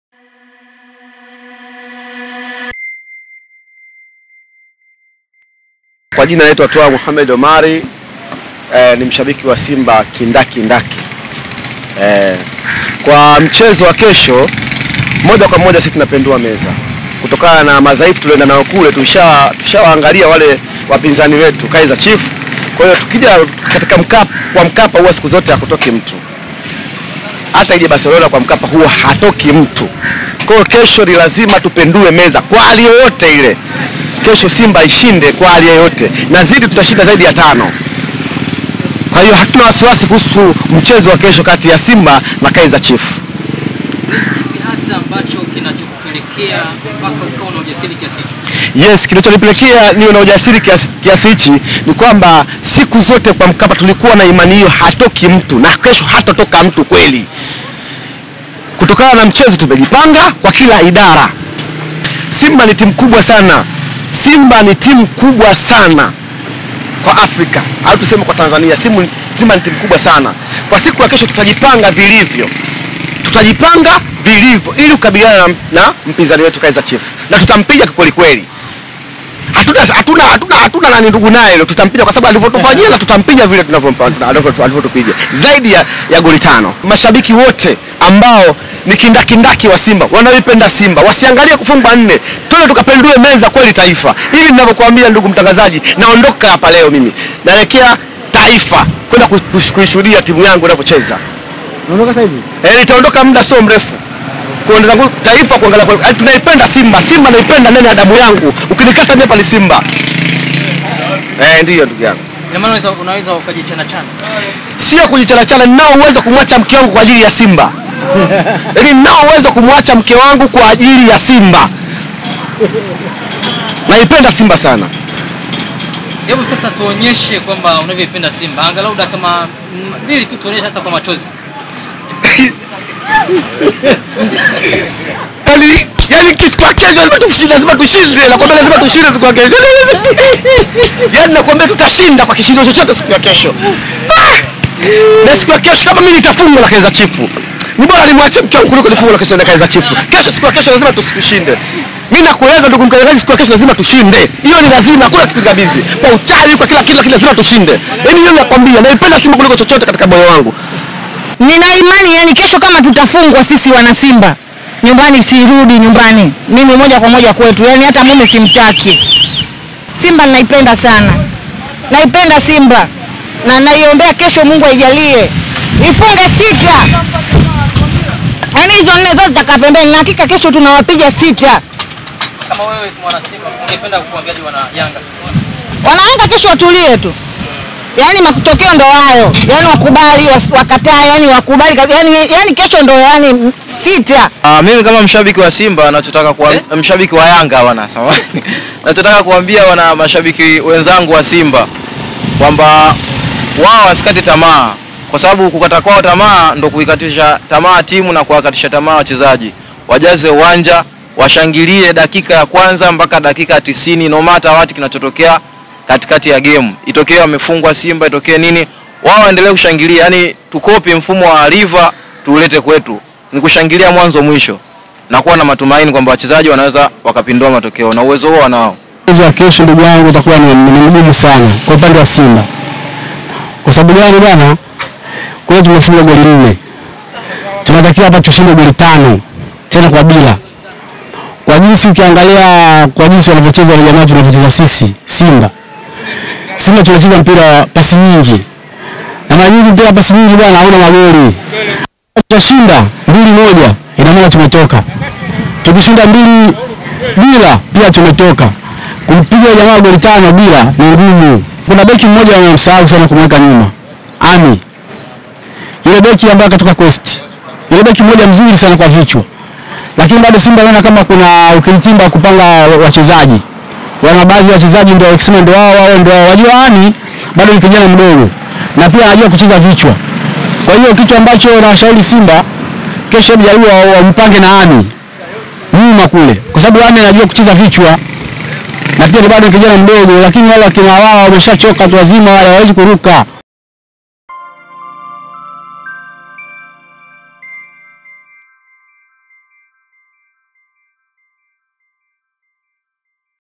KUELEKEA MCHEZO WA SIMBA Vs KAIZER CHIEF MKAPA/VIAPO VYATOLEWA TALAKA MUME NA MKE SIMBA IKIFUNGWA
Kwa Matangazo / Habari Tupigie/ Tuma ujumbe kwa namba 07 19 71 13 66